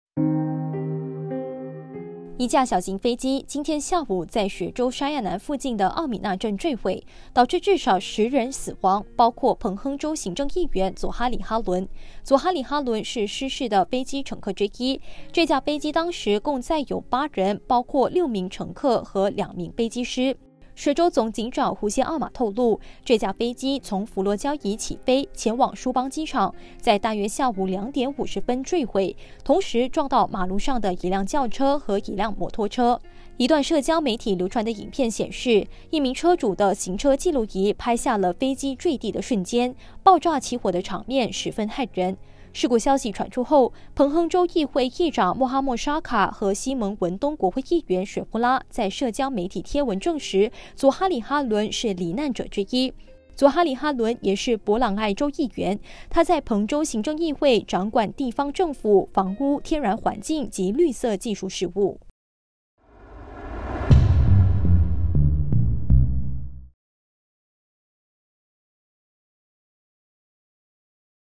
小型飞机坠毁瞬间爆炸骇人 10人罹难包括彭行政议员
一架小型飞机今天下午在雪州沙亚南附近的奥米纳镇（City of Elmina）坠毁，导致至少10人死亡，包括彭亨州行政议员佐哈里哈伦。

佐哈里哈伦是失事的飞机乘客之一。这架飞机当时共载有8人，包括6名乘客和2名飞机师。

雪州总警长胡仙奥玛透露，这架飞机从浮罗交怡起飞前往梳邦机场，在大约下午2点50分坠毁，同时撞到马路上的一辆轿车和一辆摩托车。

一段社交媒体流传的影片显示，一名车主的行车记录仪拍下了飞机坠地的瞬间，爆炸起火的场面十分骇人。

事故消息传出后，彭亨州议会议长莫哈末沙卡（Mohd Sharkar Shamsudin）和希盟文冬国会议员雪芙拉在社媒贴文证实佐哈里哈伦是罹难者之一。

佐哈里哈伦也是柏朗埃（Pelangai）州议员。他在彭州行政议会里，掌管地方政府、房屋、天然环境与绿色技术事务。